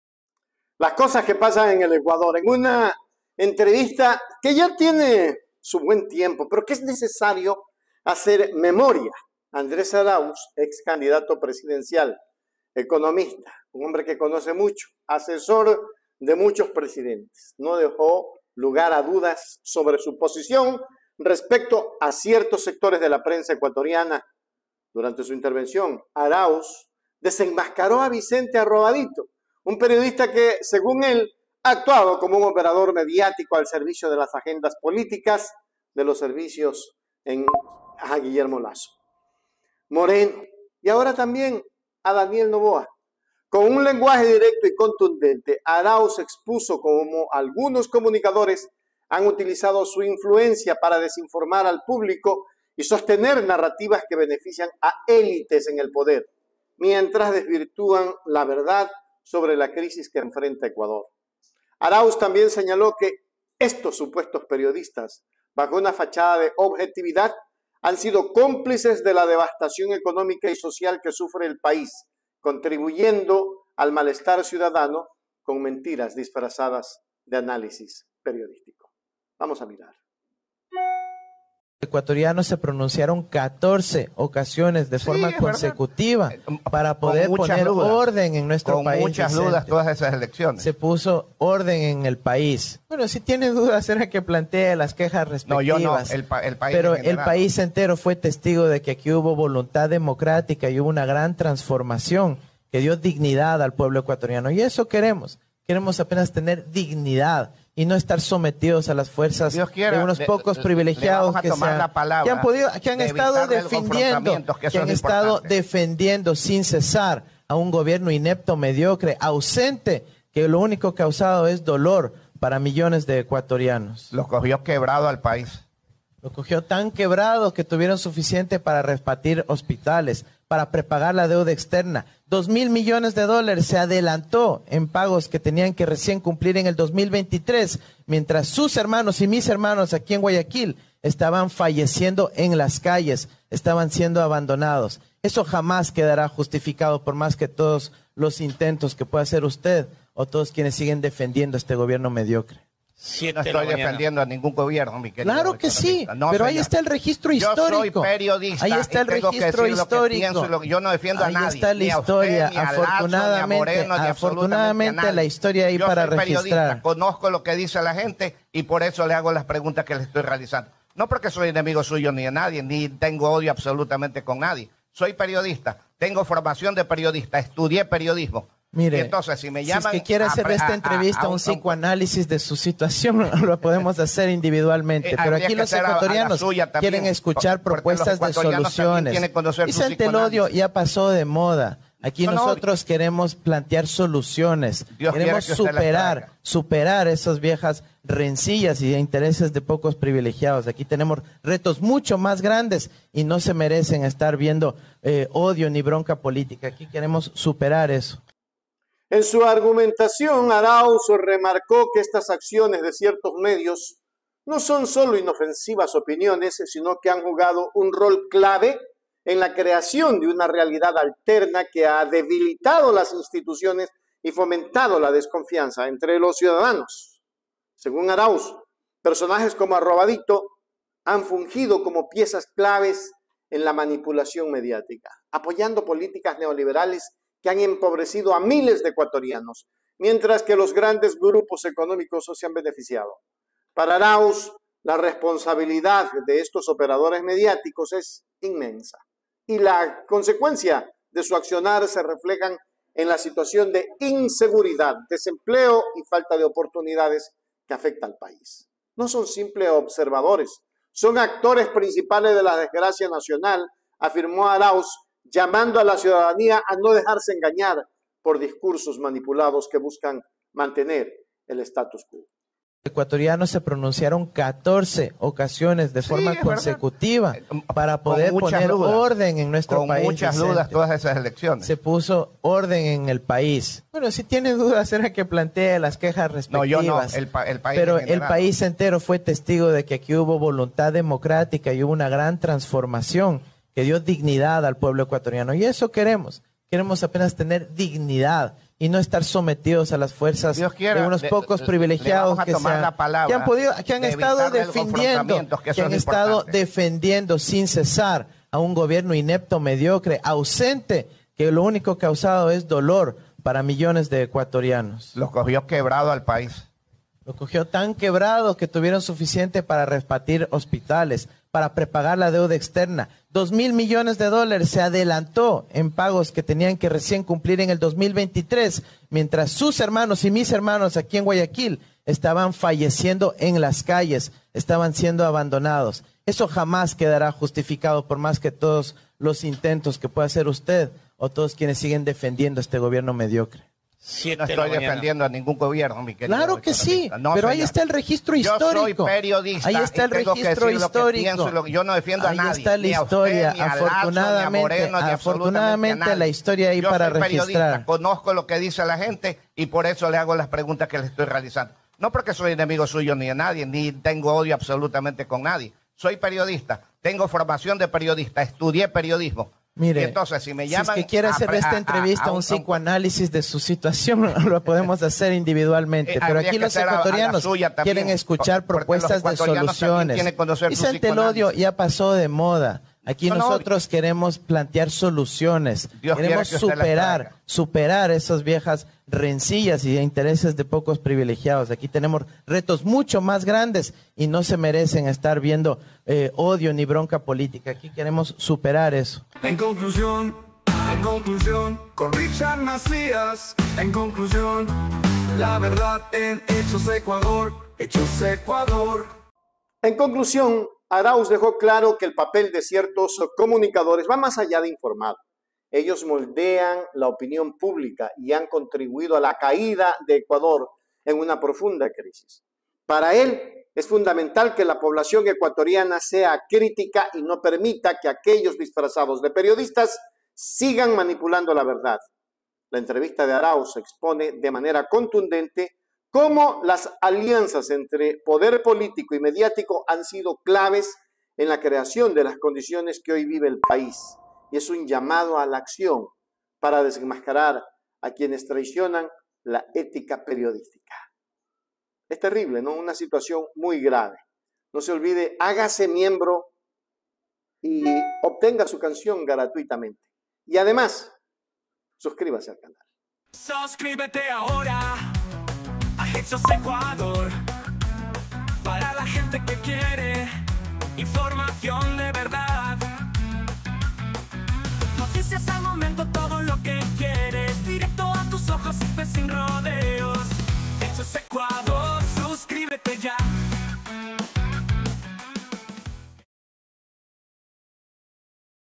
¡Andrés Arauz DESTRUYE a Vicente Arroba dito! El oscuro rol de los medios pro-Lasso, Moreno y Noboa”
Andrés Arauz no se guarda nada y desenmascara a Vicente Arrobadito, acusándolo de ser un operador mediático al servicio de los gobiernos de Lasso, Moreno, y Noboa. En esta explosiva intervención, Arauz revela cómo ciertos periodistas han manipulado la verdad y contribuido a la crisis que hoy azota al Ecuador. ¡Descubre cómo la prensa ha sido cómplice de las élites y qué significa esto para el futuro del país!